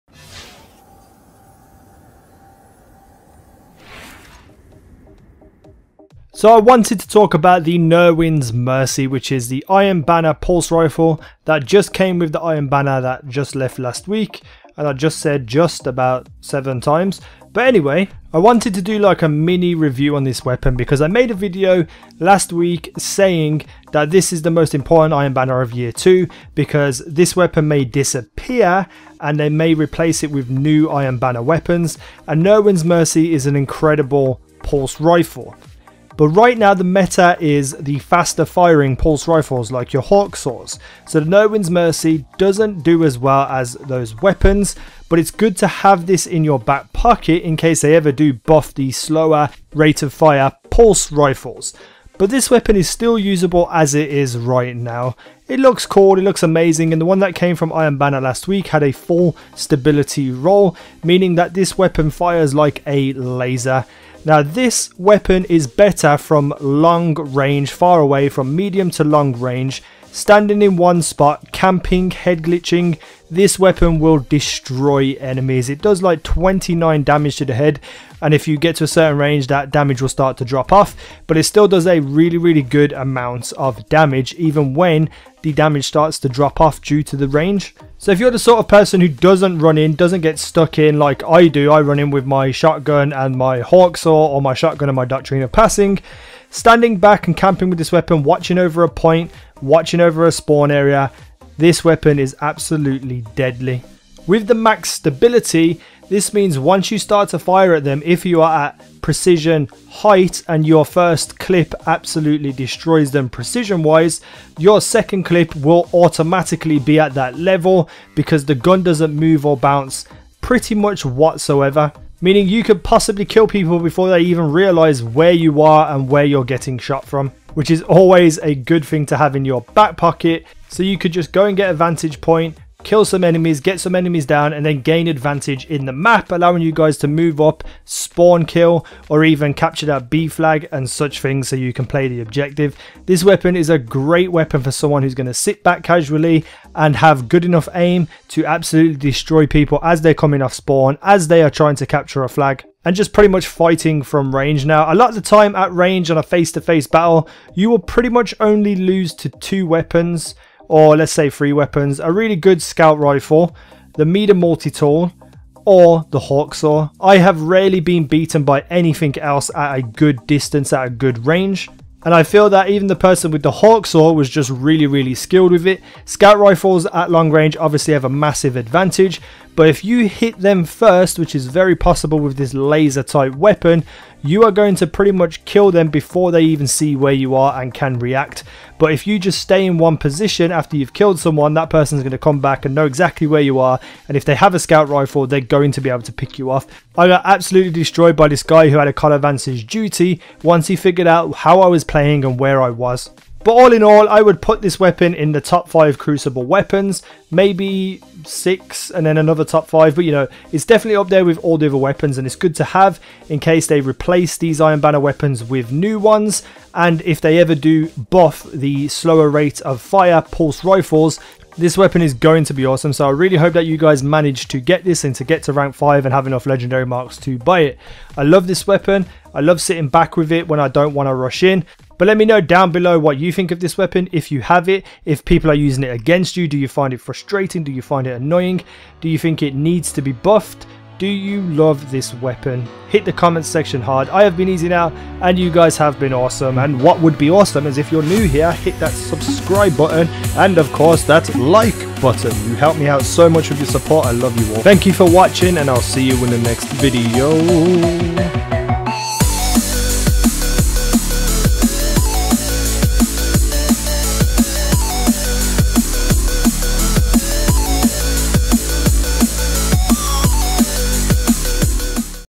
Nirwen's Mercy Review - GODLIKE PULSE RIFLE ? Or Average?!
I HAVE SPENT OVER $1000 on this game! In this "Nirwens Mercy Review" the iron banner pulse rifle, is it a "godlike pulse rifle" or is it just average?! Here are my thoughts